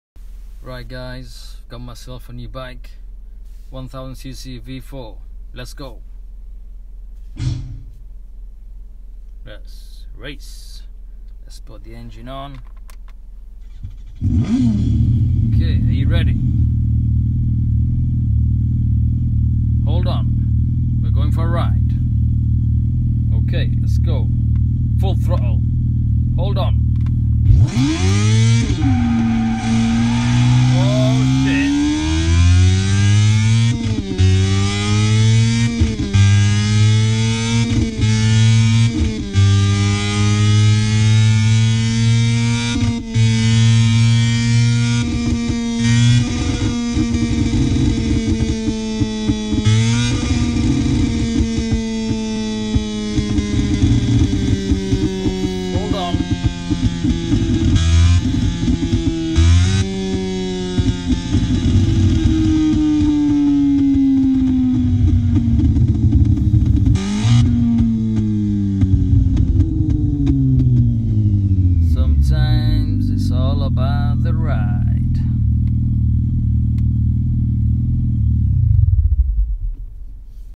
Ducati 1000cc V4 sound
V4 sound from power up to power down and down shift